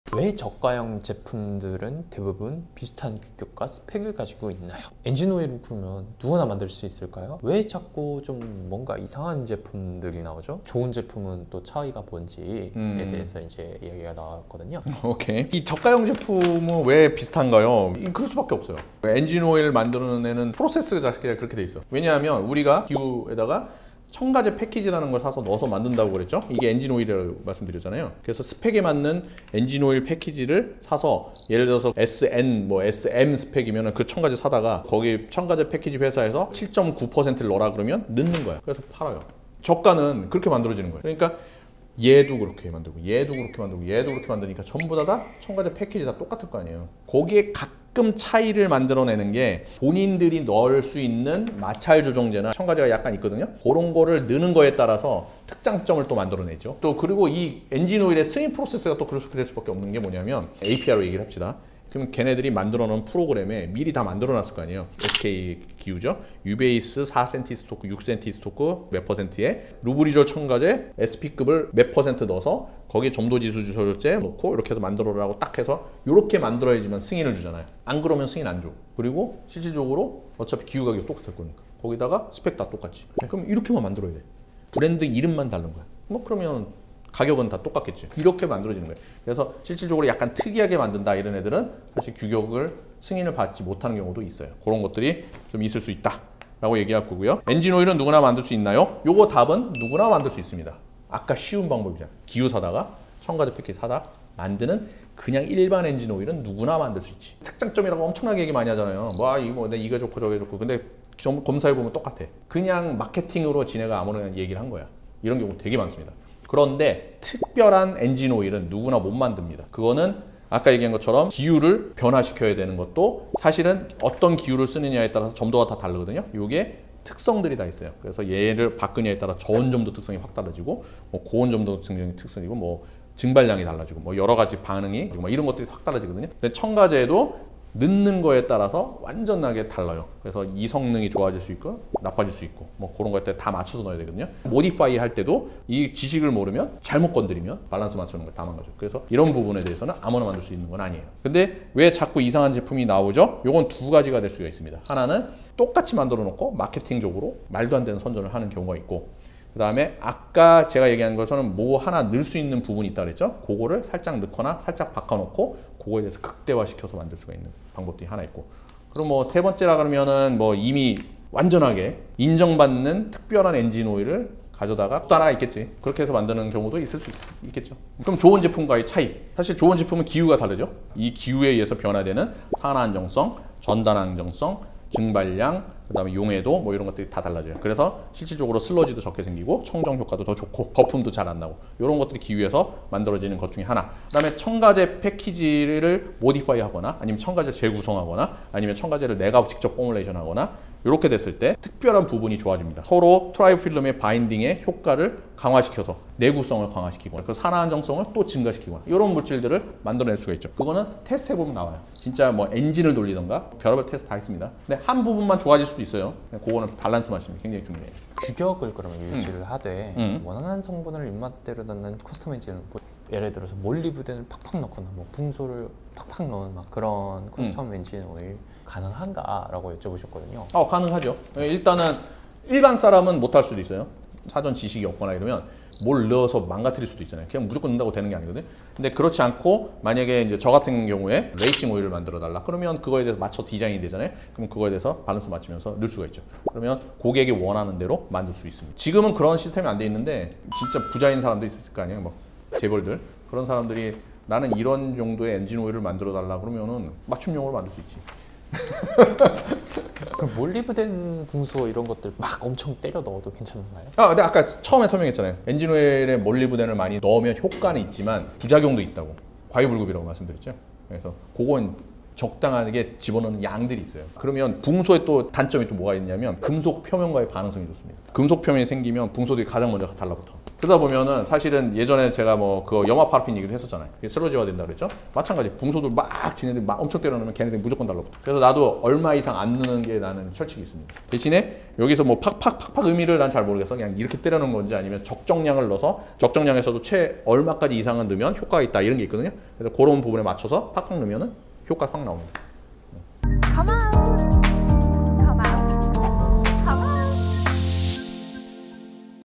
ep.77 계속 나오는 엔진오일, 믿고 사용해도 될까?
종종 보이는 이름 모를 회사의 엔진오일들에 대해서 박사님께 여쭤봤습니다!

엔진오일은 누구나 만들 수 있을까요?
저가형 제품들은 왜 비슷한 규격과 스펙을 가지고 있을까요?
정말 효과 좋은 제품도 누구나 만들 수 있는 건가요?

엔진오일에 궁금했던 것들이 있다면 영상을 시청해보세요!

타임라인
0:00 시청자의 질문!
0:18 저가형 제품은 왜 비슷한가요??
0:54 저가형 패키지 외 추가 첨가제를 넣어서…
1:25 브랜드 이름만 다르고 가격과 제품이 비슷할 수 밖에 없는 이유!
1:39 그럼 엔진오일은 누구나 만들 수 있는 건가요?
1:58 특별한 엔진오일도?!
2:30 왜 자꾸 비슷한 제품이 나오는 걸까요?
3:40 커스터마이징 오일은 가능한가요?
4:20 몰리브덴, 붕소를 많이 넣어도 괜찮은가요?

✔진짜 전문가가 만든 "엔진오일 첨가제", 리스타 S1

✔ 혁신적인 프리미엄 롱라이프 엔진오일 "리스타 메탈로센"